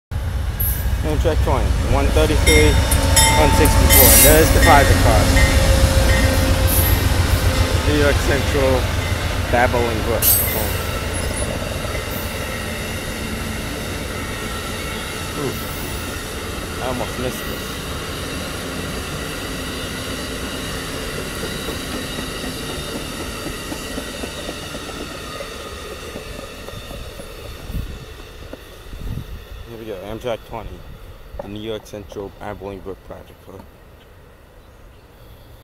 Amtrak 20 with the New York Central Babbling Brook private car in Charlottesville Virginia
The car needs to be turned around and be put at the rear.

Train info
Symbol: Amtrak 20, NS 020
Type of train: Amtrak Crescent (Passenger)
Date and time: 4/30/21 8:55 am
Origin: New Orleans, LA
Destination: New York, NY
Frequency: Originates Tuesday, Thursday and Sunday but I see it Wednesday, Friday and Monday